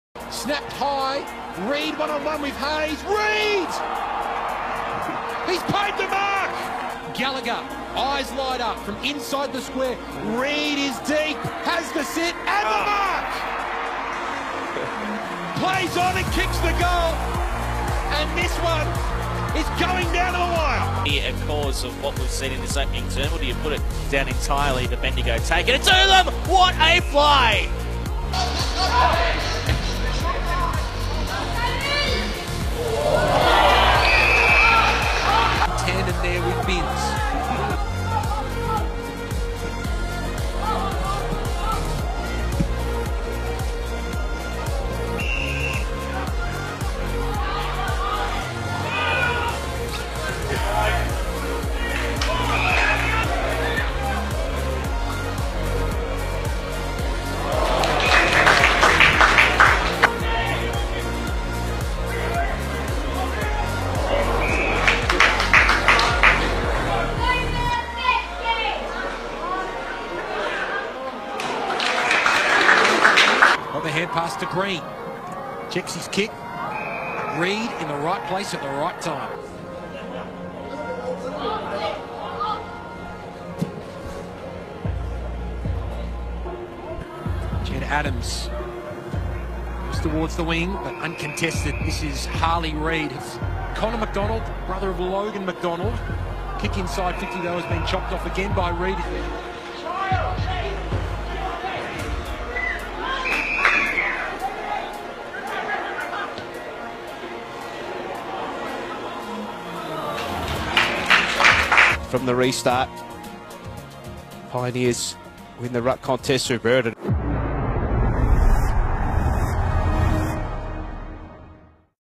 2023 AFL Draft Pick #1 - Harley Reid
Check out the highlights from our newest Eagle!